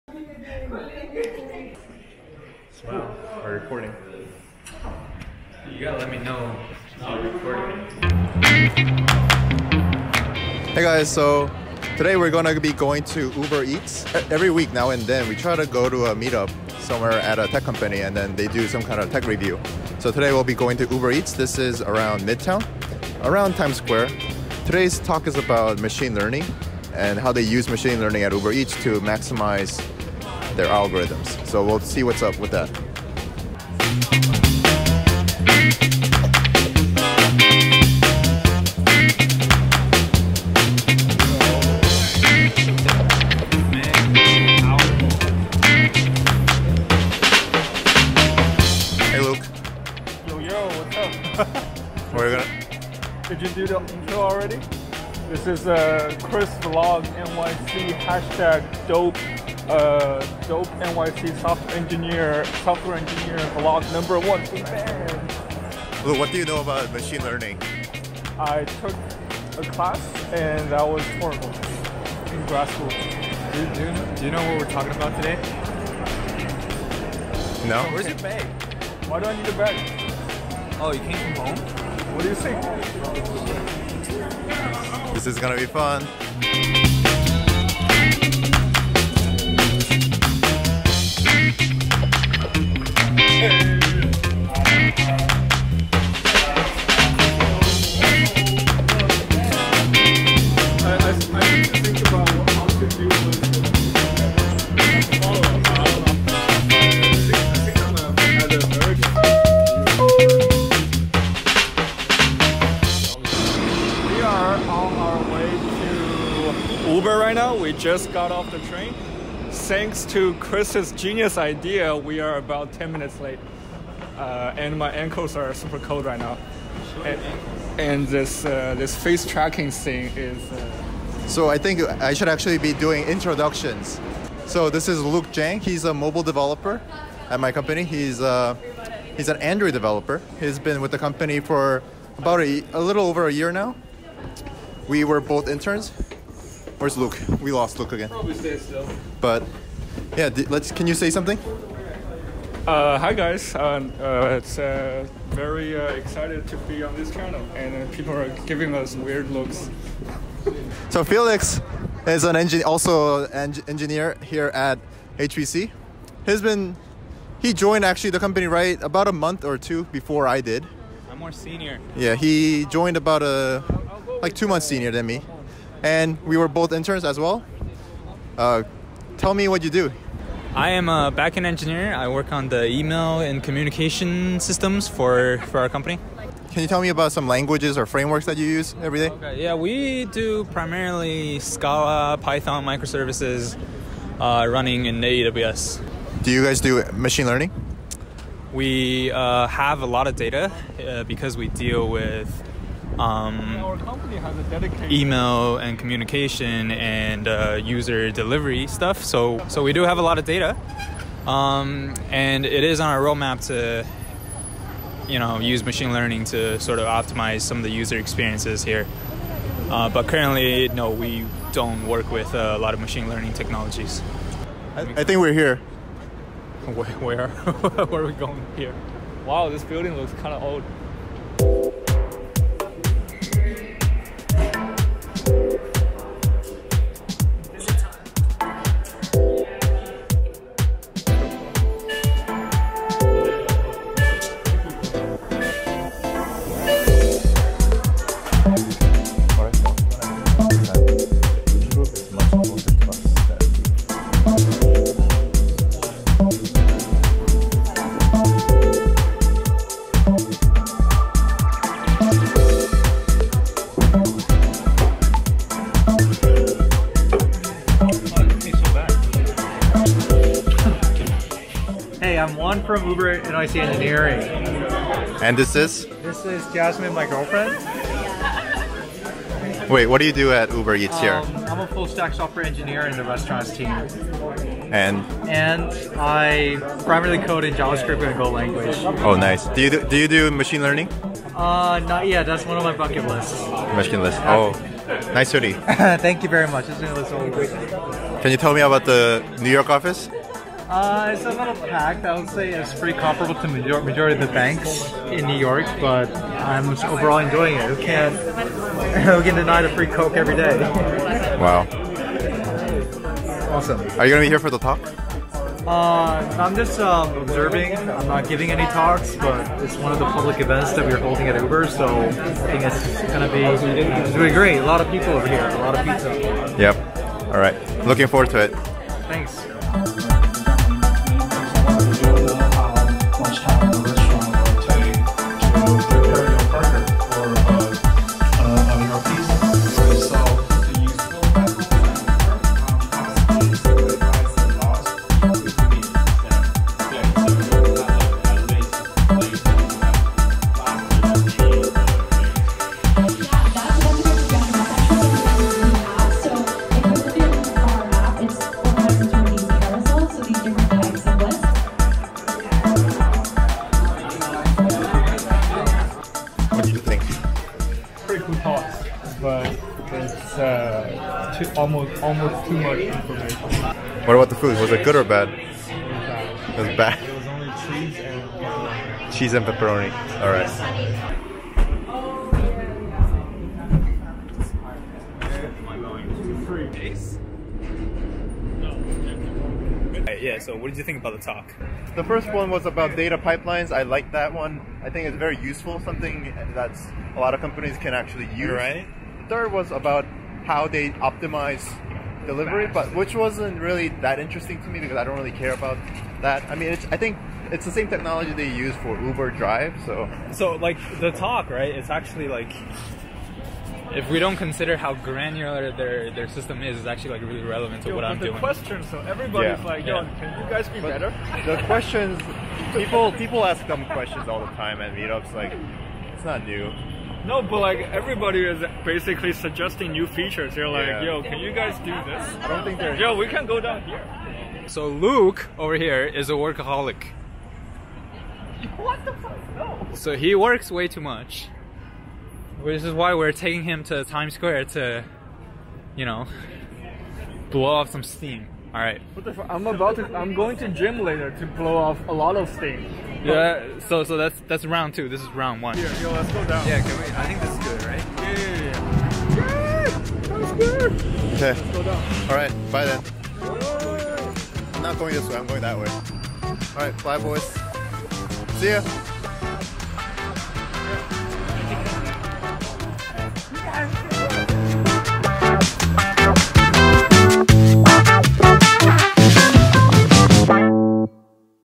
[vlog] Machine Learning Meet Up at Uber Eats
Today Luke, Felix and I headed to Uber Eats' office in Midtown NYC. We found the event on meetup.com. The topic was on how Uber Eats currently use machine learning to optimize their user experience.

First talk about about data pipelines and how they gather and congest that data. Some tools mentioned were Apache Kafka to have multiple application sending data and consuming streams out of it.

Next talk was about how UE use their in house built tool famously known Michelangelo to get an estimate of the delivery time of the meal. They use previous order history to get estimates of what a particular meal takes to prepare, then efficiently notify the partner to arrive at the restaurant on time.

Last talk was about how Uber Eats use ML to optimize the actual delivery process. Some examples and experiments were by using batch order processing. One partner would pick up two different meals from the same restaurant. Then another method of heading to two different restaurants with a similar destination.